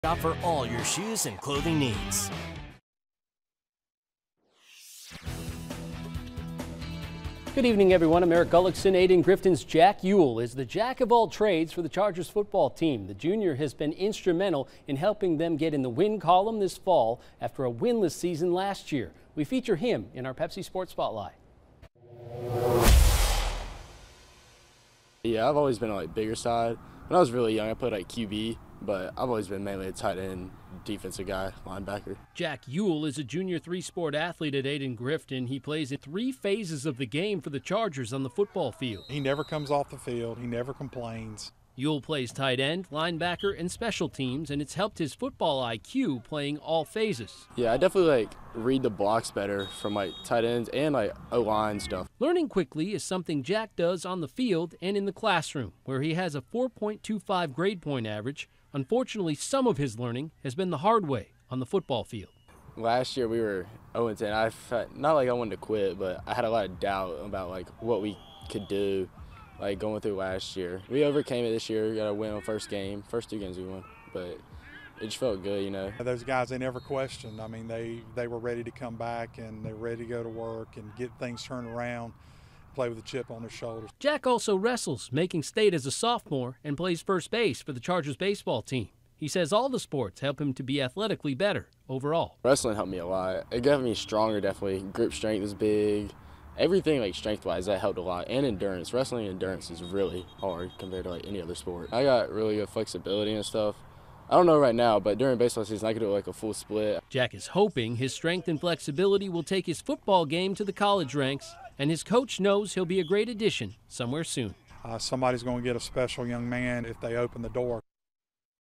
Sports Spotlight: Ayden-Grifton’s Jack Ewell seeing gains from playing three sports, sticking wit...
Sports Spotlight: Ayden-Grifton’s Jack Ewell seeing gains from playing three sports, sticking with football after winless season